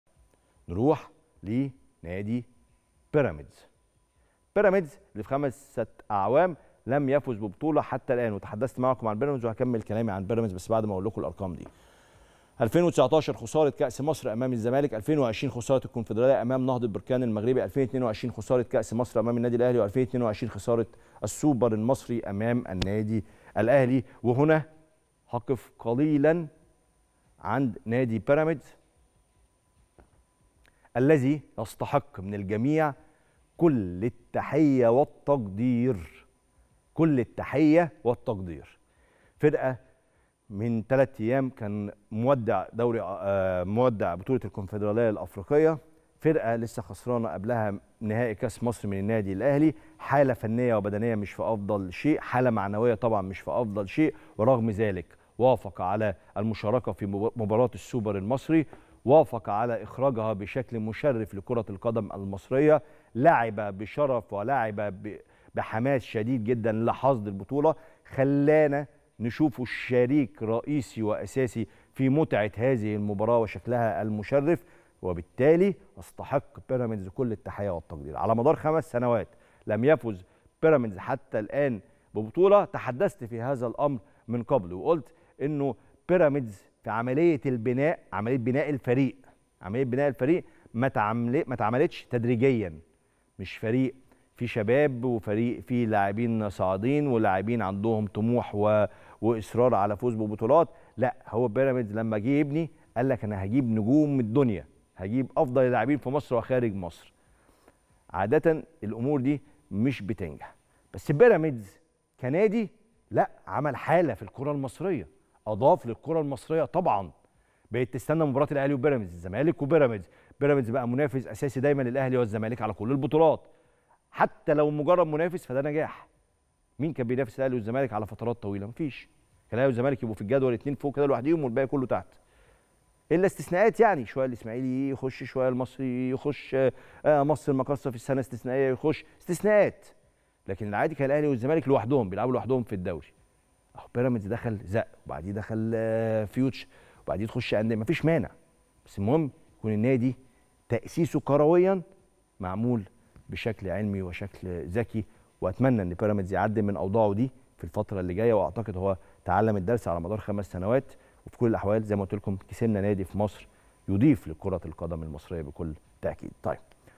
كريم رمزي يستعرض البطولات التي خسرهابيراميدز في النهائي منذ تأسيسه ورغم ذلك عمل حالة في الكرةالمصرية
كريم رمزي يستعرض البطولات التي خسرها بيراميدز في النهائي منذ تأسيسه 🔥🔥ورغم ذلك عمل حالة في الكرة المصرية 👇🏻